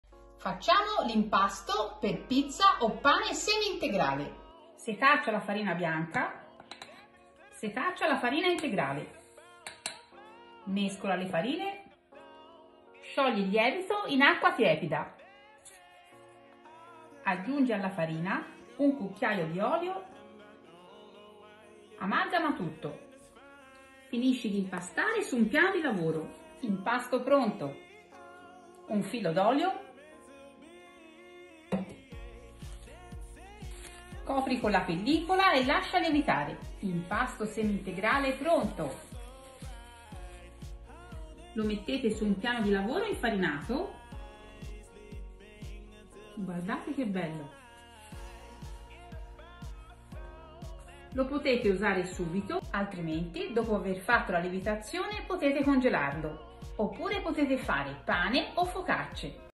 Impasto Semi-Integrale Per Pizza, Pane o Focaccia Ricetta Facile
Impasto semi integrale per pizza, pane o focaccia

Ingredienti:
300 g farina 0 setacciata ( o mix pizza)
200 g farina integrale setacciata
7 g di lievito secco per pizza e salati (1 bustina)
1 cucchiaio d’olio extravergine d’oliva
Acqua 290 g

Far lievitare l’impasto fino al raddoppio, far lievitare 3/4 ore, se avete tempo anche di più, poi formate i panetti e tenete coperto l’impasto per un altra ora, poi condire ed infornare. Oppure una volta pronta la lievitazione potete dividere in panetti da 220 g l’uno e congelare, al momento dell’utilizzo scongelare tenendoli in frigo e successivamente a temperatura ambiente, condire a piacere ed infornare. In forno preriscaldato ventilato 220 gradi per 15/20 minuti circa